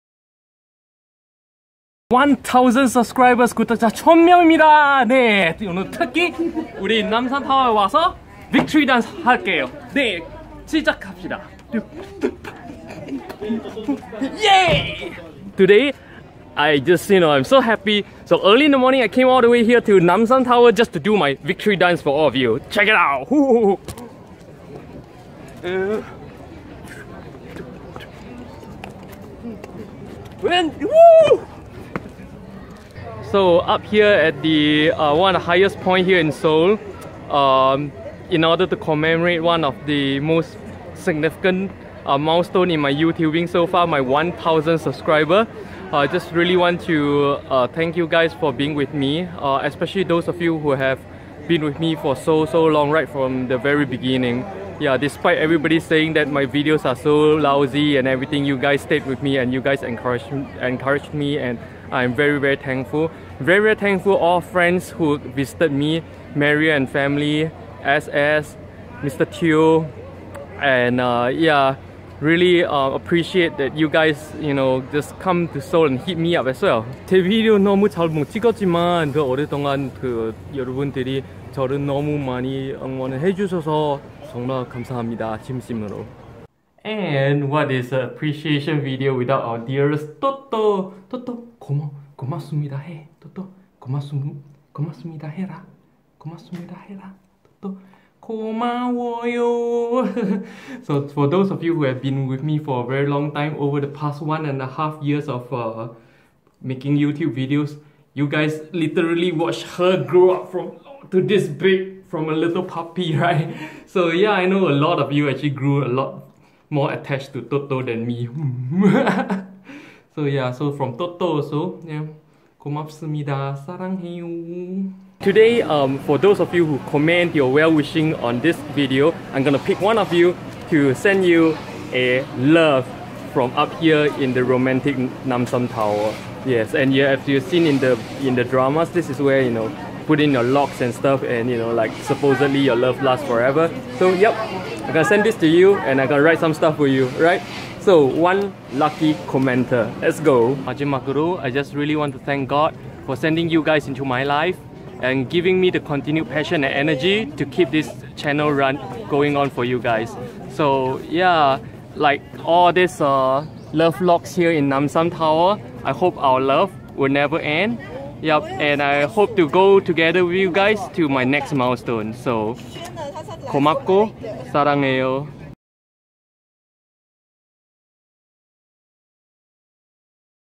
1000 Subscribers! 구독자 1000명! Victory Dance @ Namsan Tower 남산 타워 N-Tower!
Thank you guys so much for all the support as I do the silliest victory dance in public in front of laughing crowds right here on Namsan Tower or N-Tower 남산타워 of Seoul! This is truly an important milestone in the lives of all serious youtubers and from this point onwards, I am going to be improving the quality of the production, better thematic video schedules made with the same spirit of showing you the fun and real Korea while debunking the fantasies all in a nice fun spirit!! Yes, my earlier videos tends to be too gloomy and I am truly sorry for that. As I evolve into this new milestone, I hope to be able to present some of the raw truths of Korea also in positive and fun manner. I hope all of you give me your support, love and feedback as we move together into our next milestone: 5000 subscribers, together!